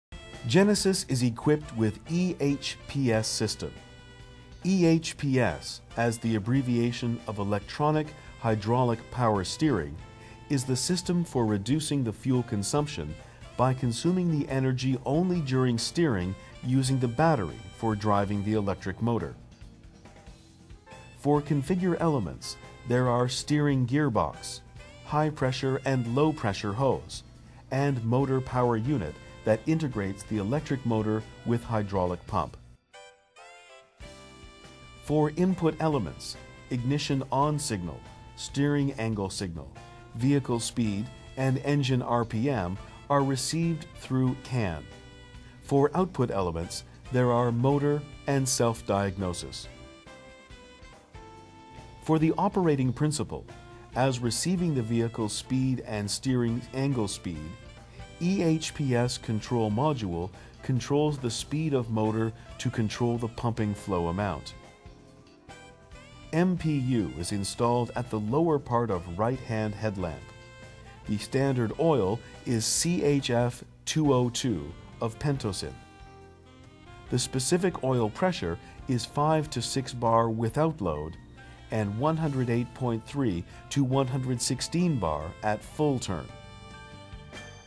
How Electric Hydraulic Power Steering Works on Hyundai Genesis (BH)
This video shows you How Electric Hydraulic Power Steering Works on Hyundai Genesis (BH)